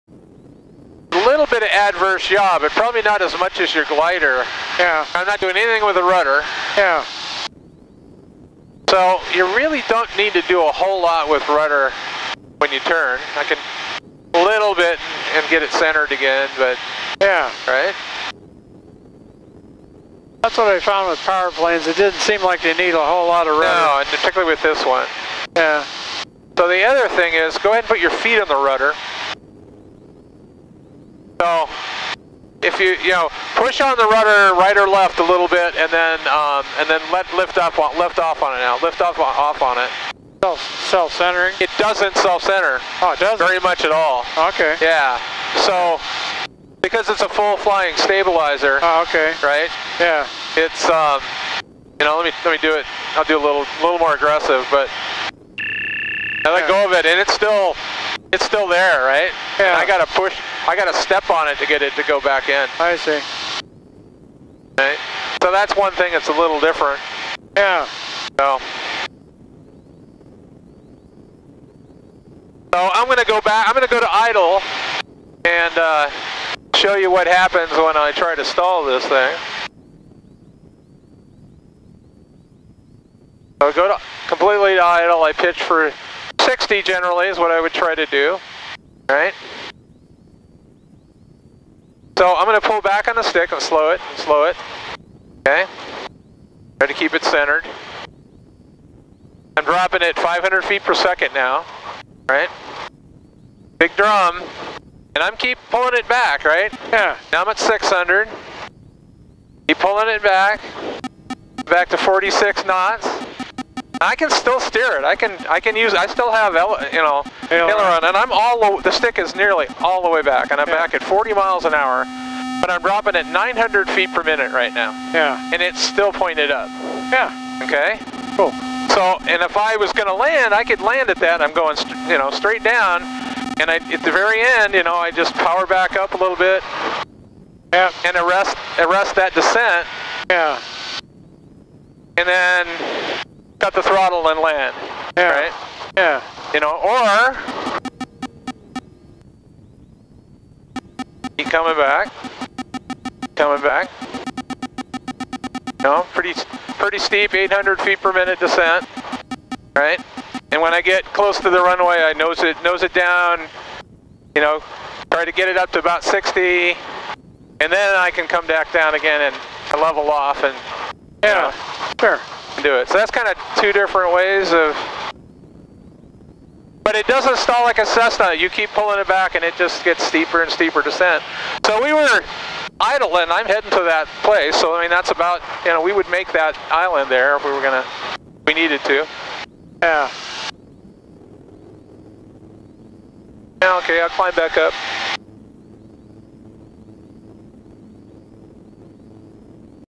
CH701 (stall) Demo
With my friend Tom Udd (glider pilot) showing what happens when I stall my Zenith CH701 (9/25/2016).